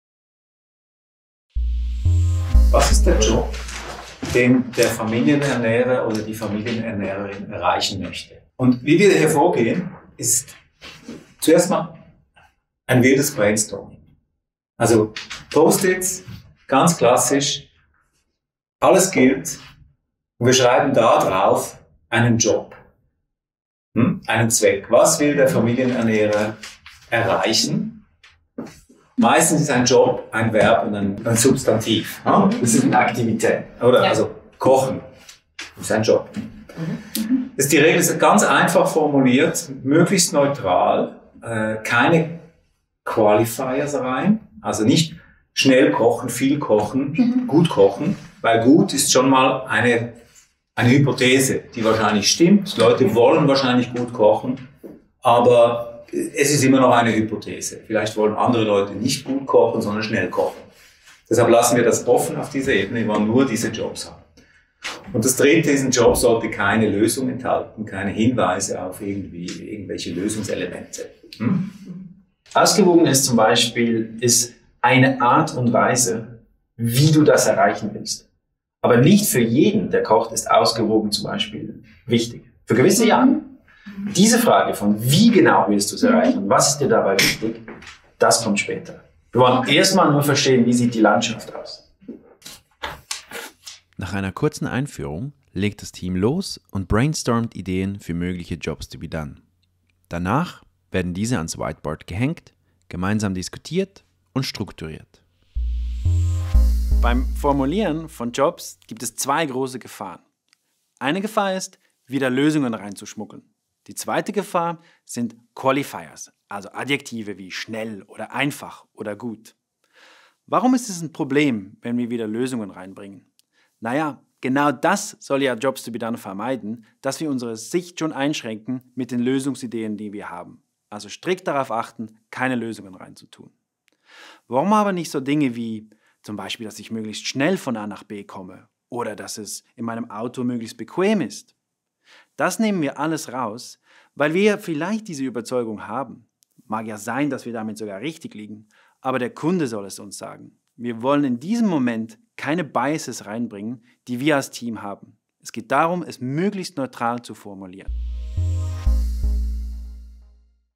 Methode: Jobs to be Done formulieren
Das Team entwickelt erste Jobs to be Done für ein Innovationsprojekt. Es geht darum, möglichst neutral die Aktivitäten zu beschreiben, die die Nutzer:innen vornehmen, um ein Ziel zu erreichen.